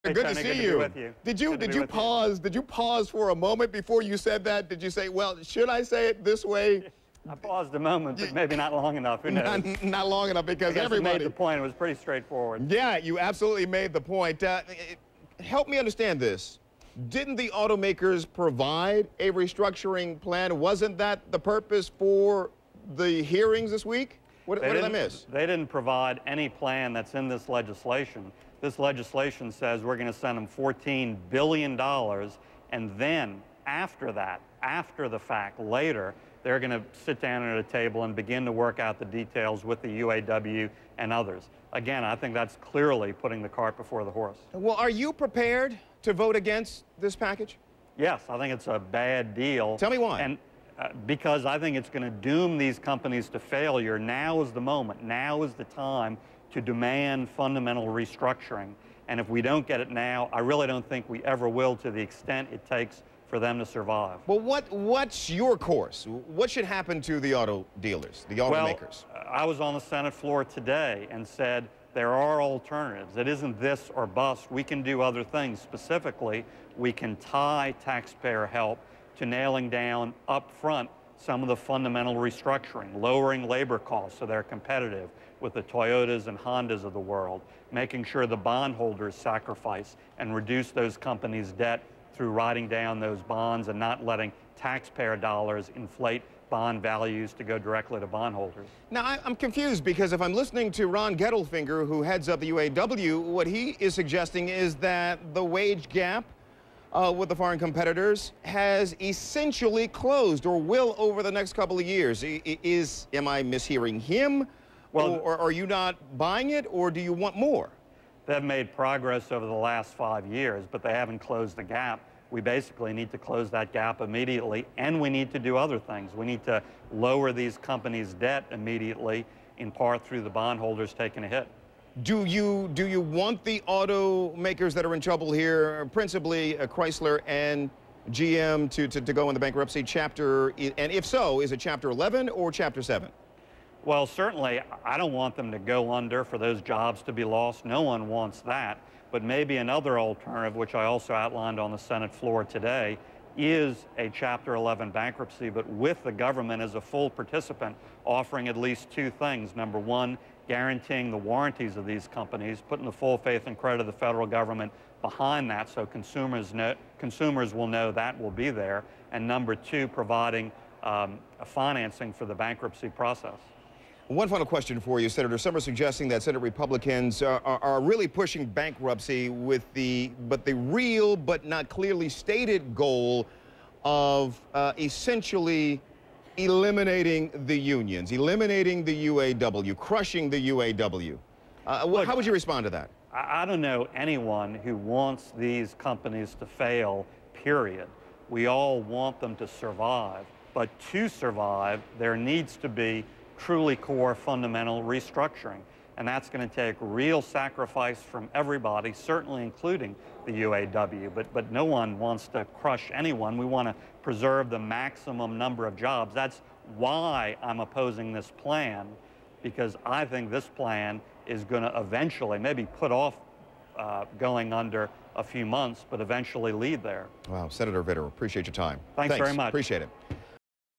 Senator David Vitter on CNN about his auto bailout opposition
louisiana's junior senator david vitter (r) explains why he opposes the auto bailout bill H.R.7321